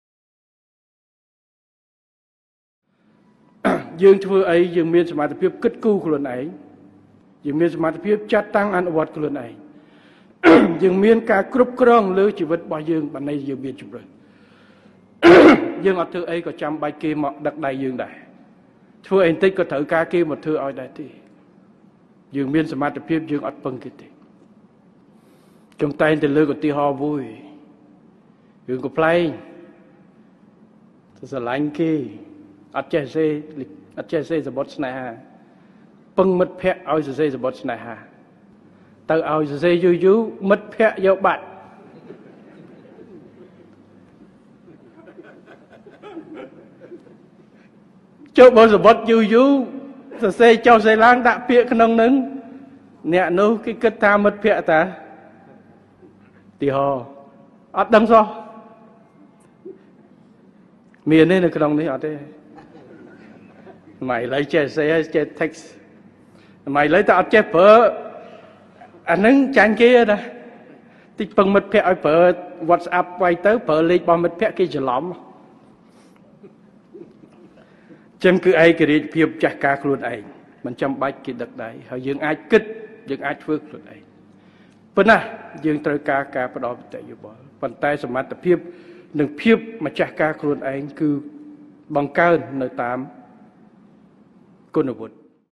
យើងត្រូវមានភាពឯករាជ្យម្ចាស់ការលើខ្លួនឯង និងរឹងមាំ ដោយមិនត្រូវពឹងគេនោះទេ
ប្រសាសន៍ សម្តេចធិបតី ​ ក្នុងពិធី “ប្រគល់សញ្ញាបត្រជូនដល់និស្សិតនៃសាកលវិទ្យាល័យ លីមកុងវីង”​ ។

រាជធានីភ្នំពេញ ១៣ មិថុនា ២០២៤